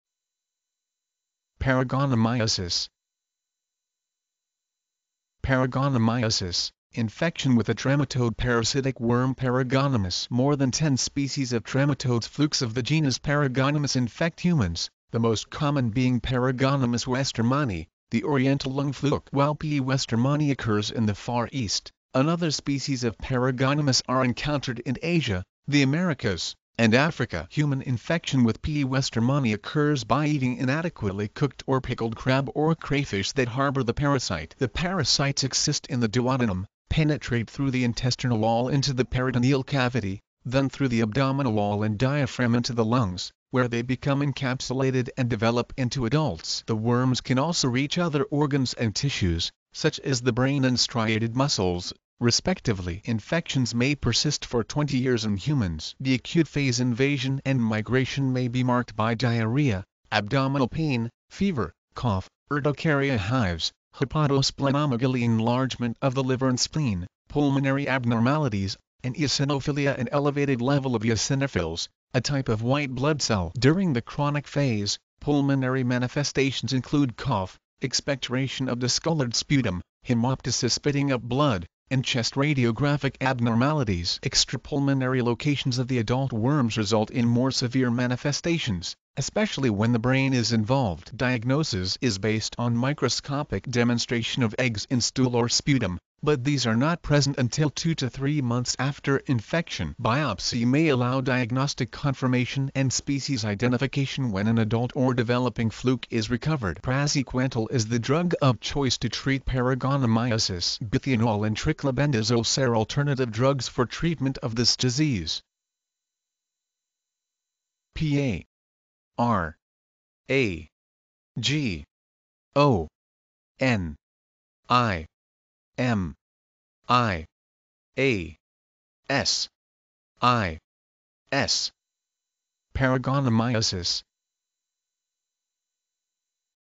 Paragonimiasis - Medical Definition
Paragonimiasis

Paragonimiasis: Infection with the trematode (parasitic worm) Paragonimus. More than 10 species of trematodes (flukes) of the genus Paragonimus infect humans, the most common being Paragonimus westermani, the oriental lung fluke. While P. westermani occurs in the Far East, other species of Paragonimus are encountered in Asia, the Americas, and Africa. Human infection with P. westermani occurs by eating inadequately cooked or pickled crab or crayfish that harbor the parasite. The parasites excyst in the duodenum, penetrate through the intestinal wall into the peritoneal cavity, then through the abdominal wall and diaphragm into the lungs, where they become encapsulated and develop into adults. The worms can also reach other organs and tissues, such as the brain and striated muscles, respectively. Infections may persist for 20 years in humans. The acute phase (invasion and migration) may be marked by diarrhea, abdominal pain, fever, cough, urticaria (hives), hepatosplenomegaly (enlargement of the liver and spleen), pulmonary abnormalities, and eosinophilia (an elevated level of eosinophils, a type of white blood cell). During the chronic phase, pulmonary manifestations include cough, expectoration of discolored sputum, hemoptysis (spitting up blood), and chest radiographic abnormalities. Extrapulmonary locations of the adult worms result in more severe manifestations, especially when the brain is involved. Diagnosis is based on microscopic demonstration of eggs in stool or sputum, but these are not present until 2 to 3 months after infection. Biopsy may allow diagnostic confirmation and species identification when an adult or developing fluke is recovered. Praziquantel is the drug of choice to treat paragonimiasis. Bithionol and triclabendazole sare alternative drugs for treatment of this disease.